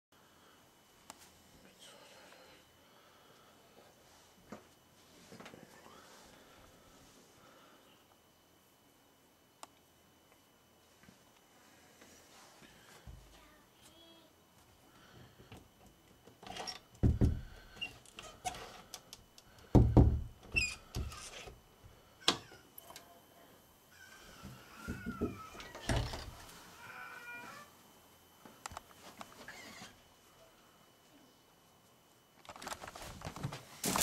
Real Paranormal Footage - Haunted House in Teesside England
A family died in this house through carbon monoxide poisoning but the couple who live in the house now say they can hear the children playing, this footage was taking by a family member who captured a girls voice and a cry after not being able to get into a bedroom the door opened to reveal a girl all caught the couple quickly left the house and now stands empty.